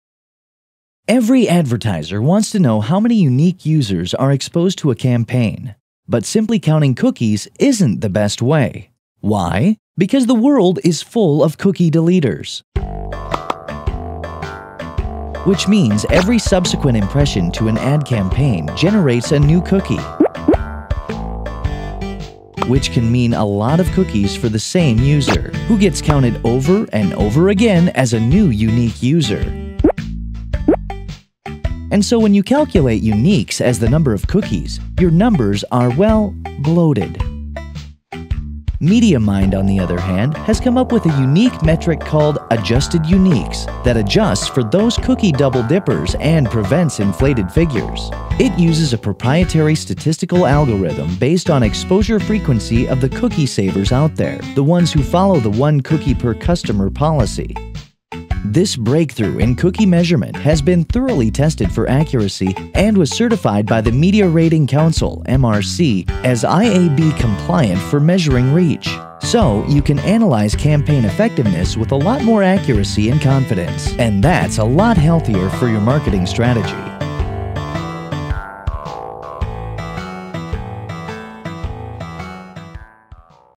Planning Audience Reach with Adjusted Unique - Chinese Subtitles
Adjusted Unique - One of the basic measures of any campaign is the number of unique users it reached.
Until now, reach measurement based on simple cookie overestimated the number of unique users due to cookie deletion. Now, MediaMind has developed Adjusted Unique, a breakthrough in online measurement that accurately measures reach, overcoming the cookie deletion issue.